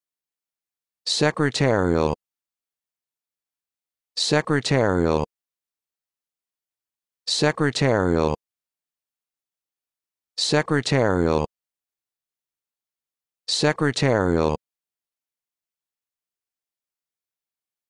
How to Pronounce Secretarial
This video shows you how to pronounce Secretarial